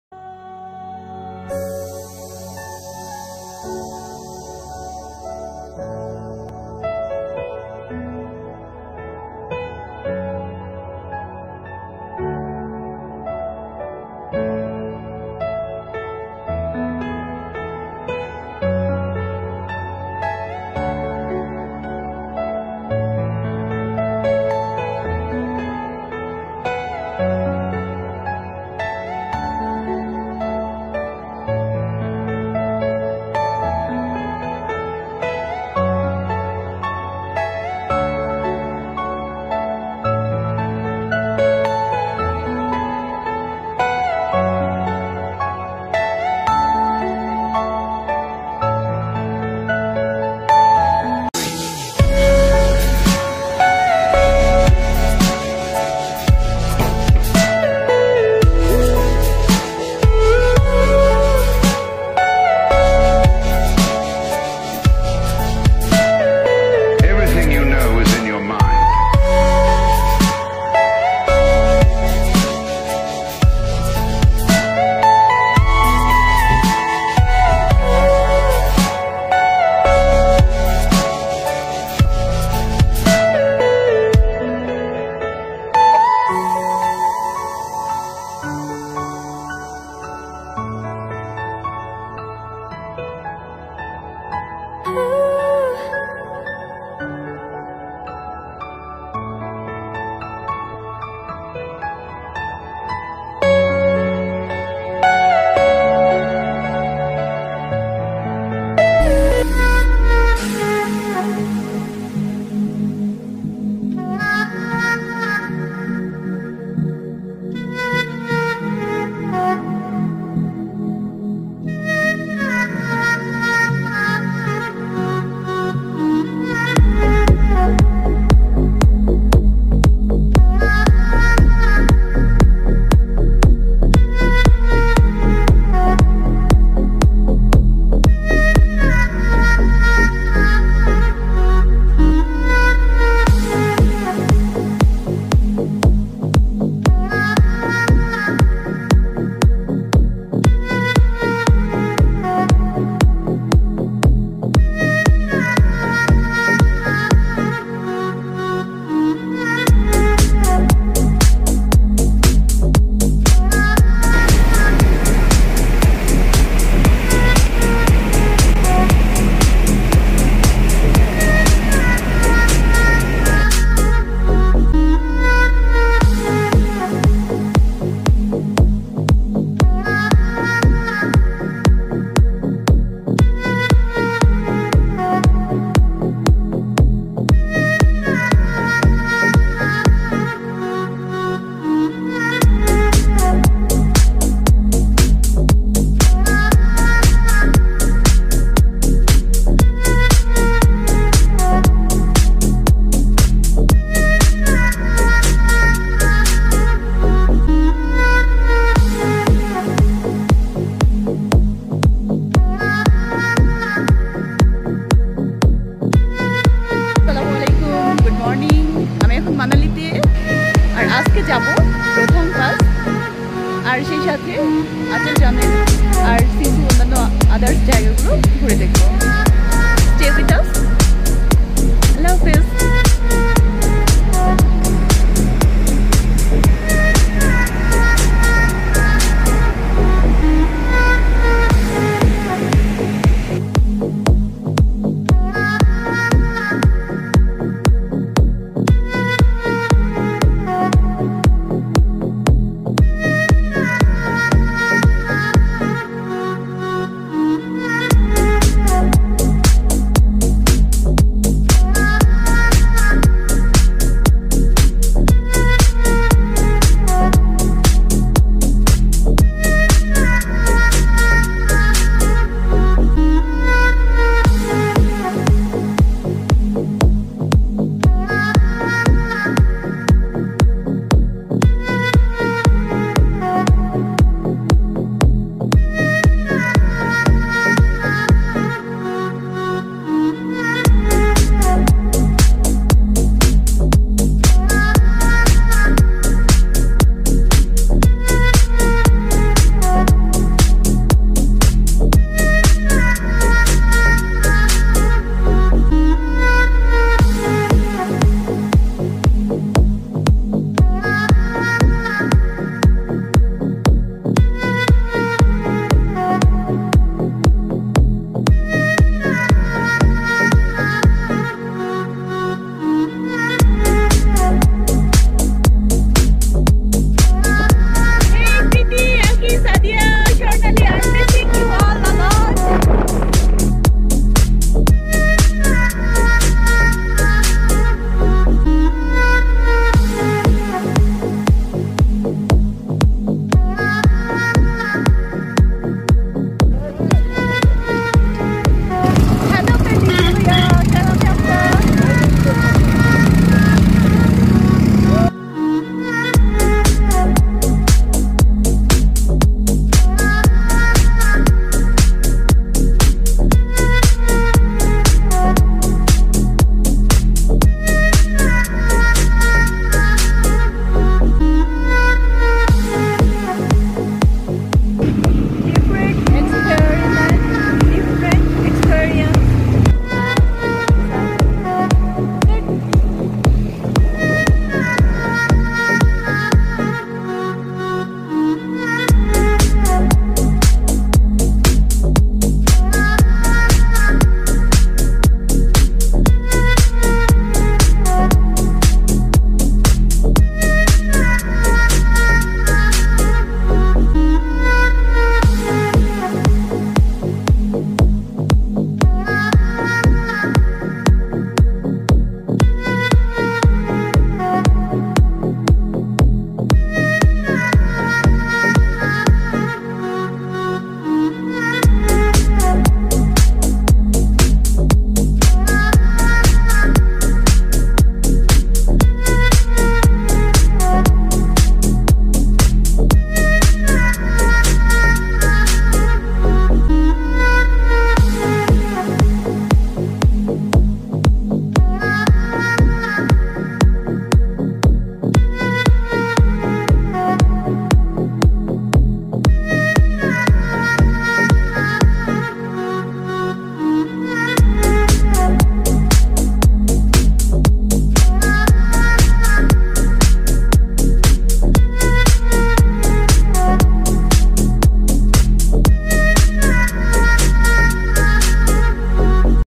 A Beautiful Journey To manali। Best time to visit Manali in June
Manali Tour। Beautiful Manali। Rajshahi to Dhaka to Delhi to Manali। Manali sightseeing